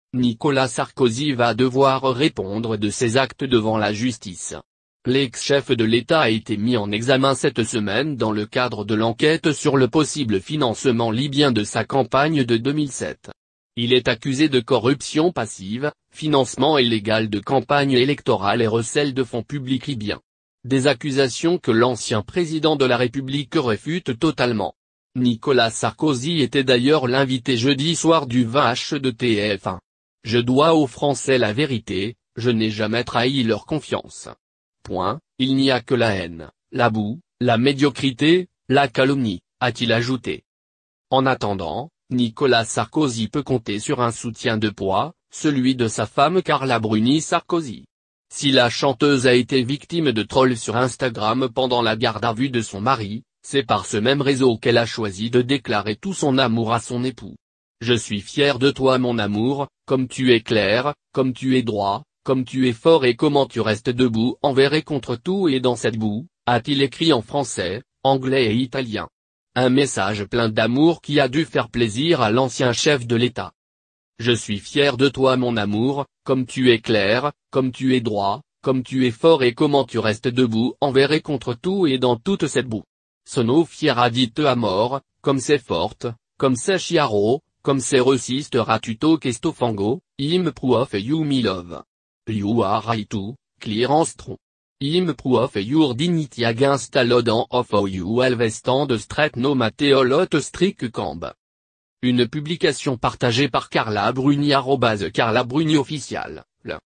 Nicolas Sarkozy mis en examen : Carla Bruni "fière de son amour" sur Instagram
Nicolas Sarkozy va devoir répondre de ses actes devant la justice. L’ex-chef de l’État a été mis en examen cette semaine dans le cadre de l’enquête sur le possible financement libyen de sa campagne de 2007. Il est accusé de "corruption passive", "financement illégal de campagne électorale" et "recel de fonds publics libyens".